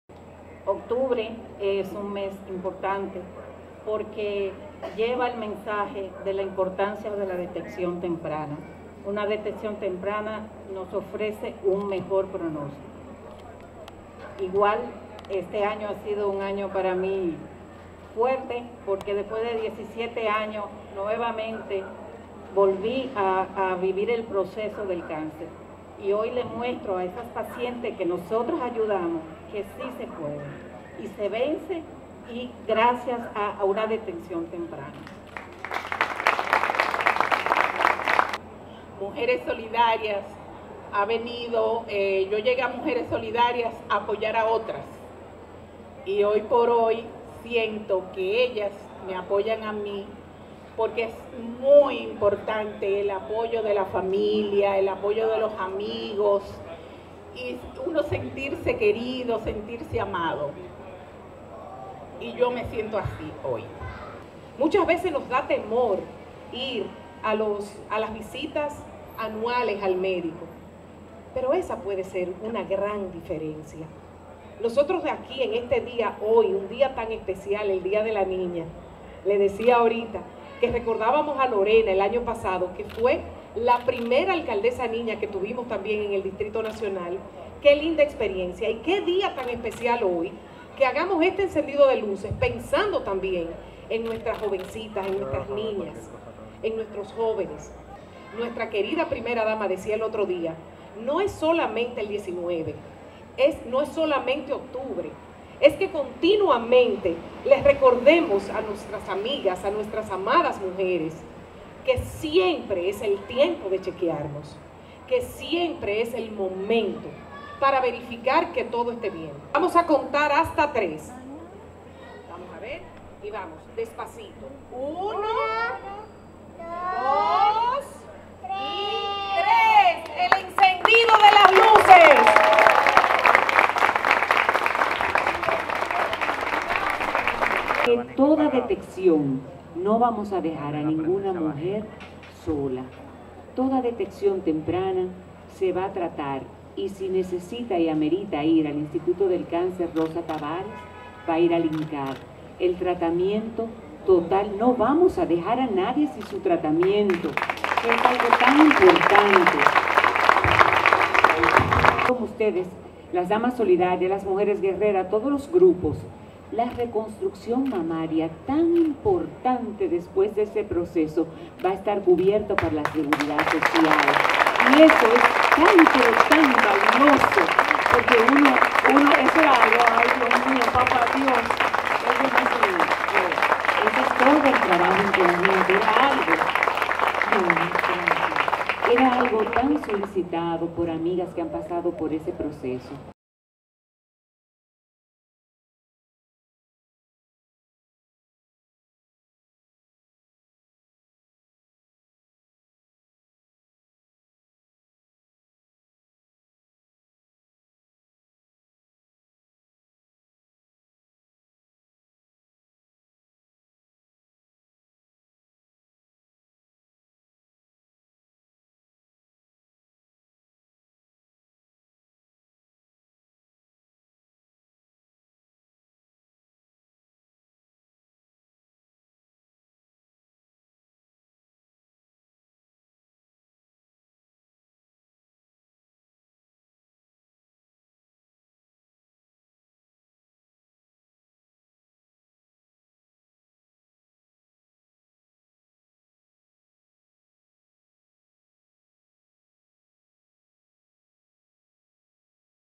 ADN ilumina de rosa Palacio Consistorial para concienciar sobre Cáncer de Mama
Santo Domingo.- En un acto encabezado por la alcaldesa Carolina Mejía y la primera dama Raquel Arbaje, la Alcaldía del Distrito Nacional (ADN) realizó el encendido de luces rosa en el Palacio Consistorial en la Ciudad Colonial en respaldo al Mes de Sensibilización contra el Cáncer de Mama.
“Este es un día muy especial.